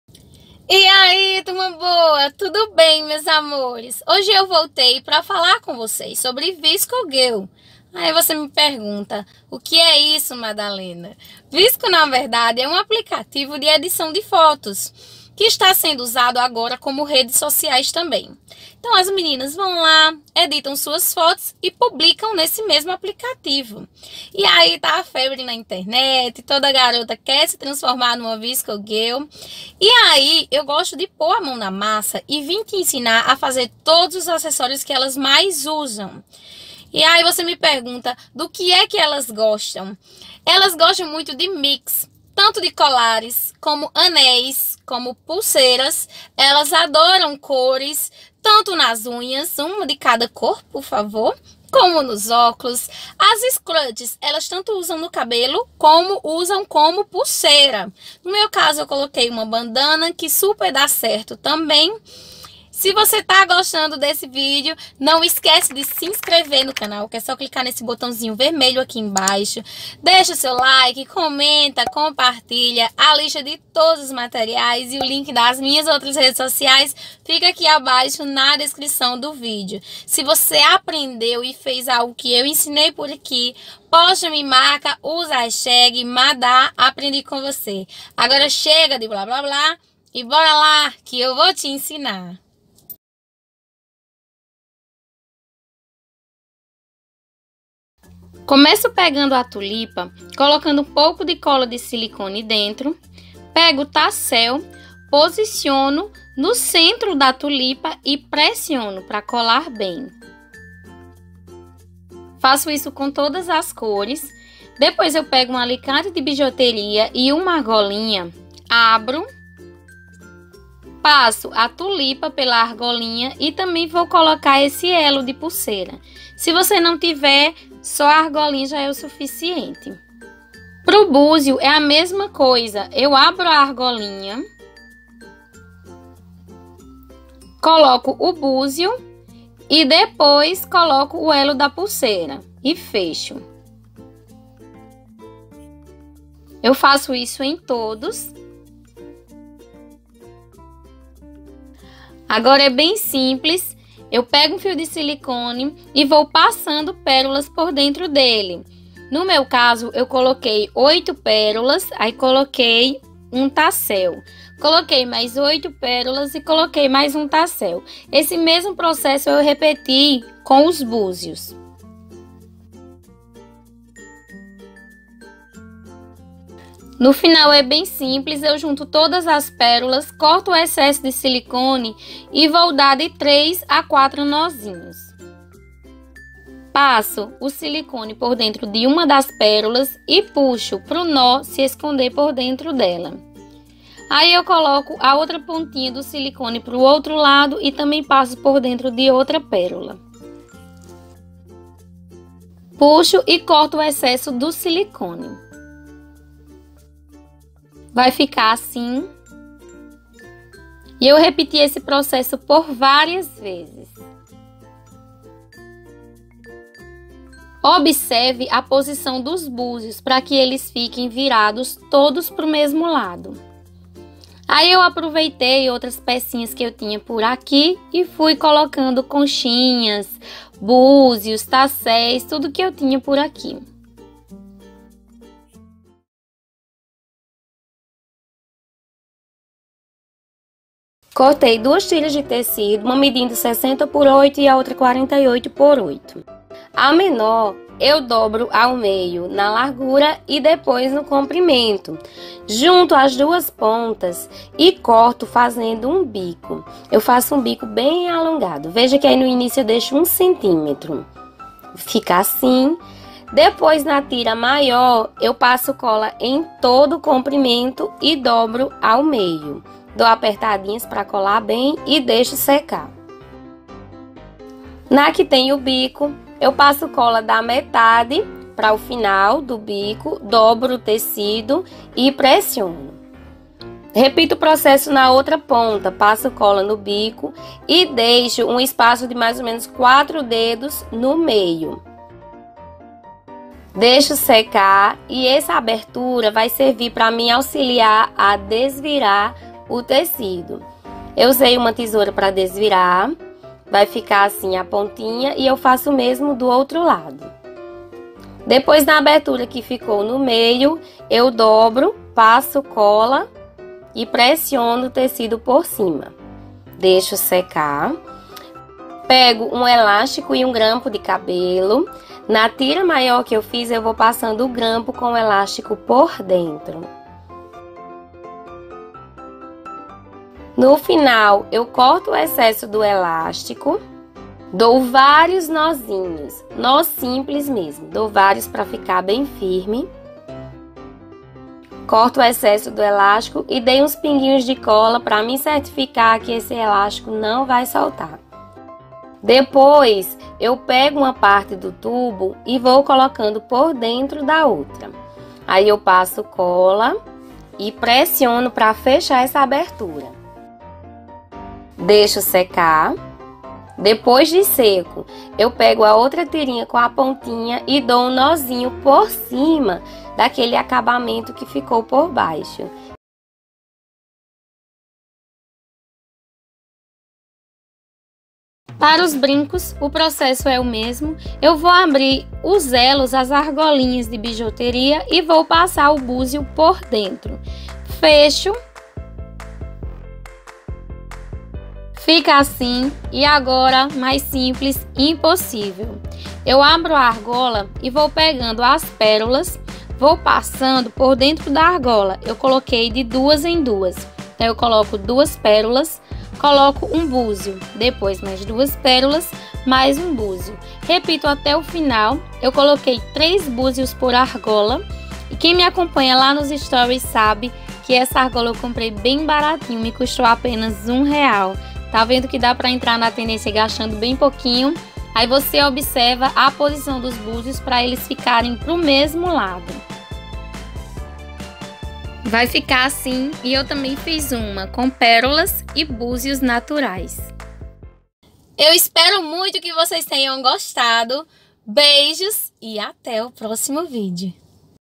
Acessórios VSCO GIRLS passo a passo
😍 E aí? Turma Boa! Hoje vim te ensinar a fazer os acessórios das VSCO GIRLS, espero que gostem! Beijos!!!

🗒 Materiais:

✏ argola
✏ pérola
✏ tecido
✏ cola pano
✏ buzio
✏ tassel
✏ fio de silicone
✏ tulipa de bijuteria
✏ passador com argalinha
✏ argolinha para bijuterias
✏ alicate
✏ tesoura

📸 Fotos -